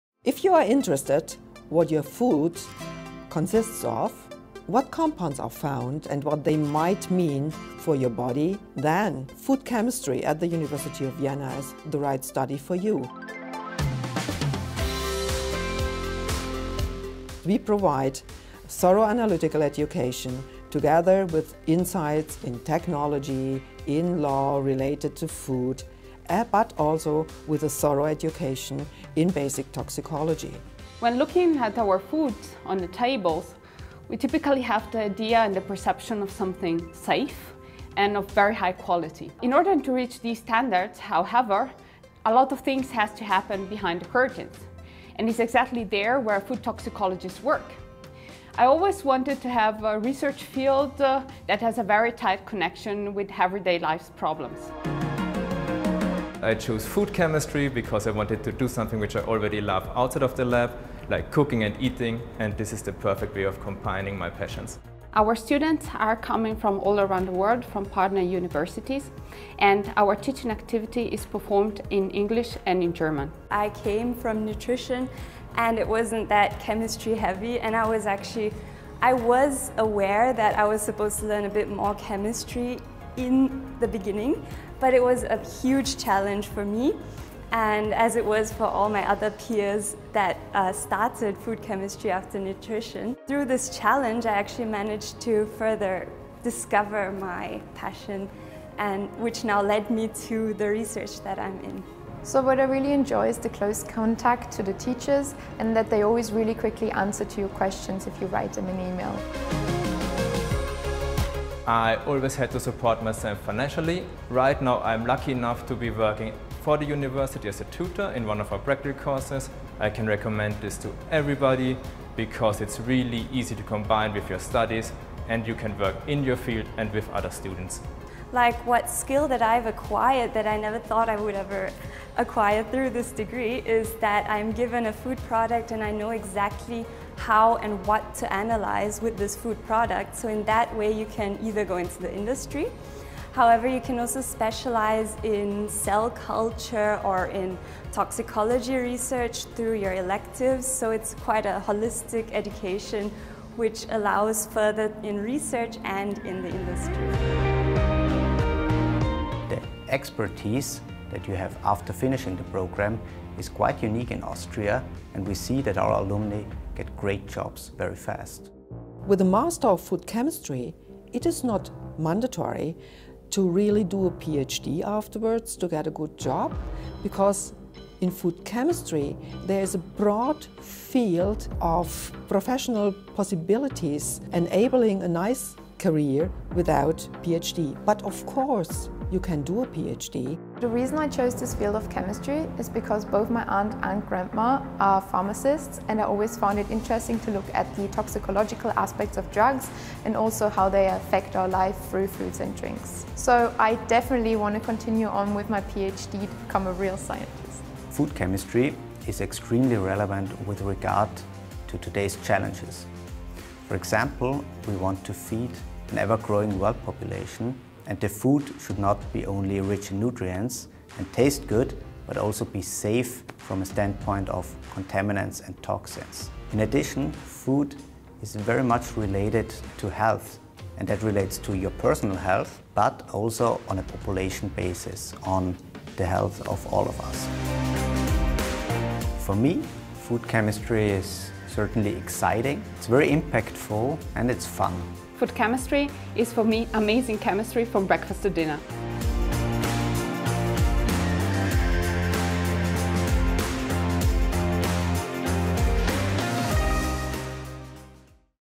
Master of Food Chemistry (MA) - University of Vienna
The video series "Considering this ..." of the Faculty of Chemistry at the University of Vienna aims at providing insights for students: What do the various study programs of chemistry offer? What are challenges? How are studying and working compatible? What are your career prospects? Teachers and students of the Faculty of Chemistry provide answers to these questions. Here we present our Master of Food Chemistry.